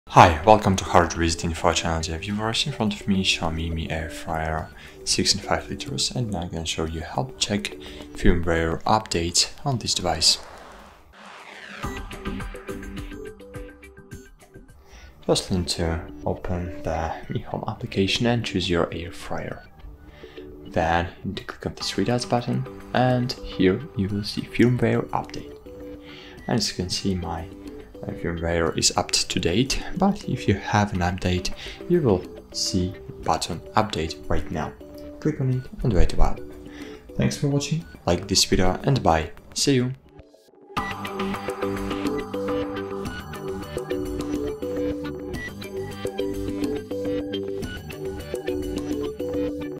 How To Check For Updates On Xiaomi Smart Airfryer
Regularly checking for updates on your Xiaomi Smart Airfryer ensures that you have the latest firmware and features, optimizing performance and functionality. In this tutorial, we'll walk you through the steps to check for updates on your Xiaomi Smart Airfryer, enabling you to stay up-to-date with the latest improvements and enhancements.

Why is it important to check for updates on the Xiaomi Smart Airfryer?
How do updates enhance the performance and functionality of the air fryer?
Can you outline the step-by-step process for checking for updates on the Xiaomi Smart Airfryer?
Are there specific settings or options users need to access to perform this check?
What indicators suggest that an update is available for the air fryer?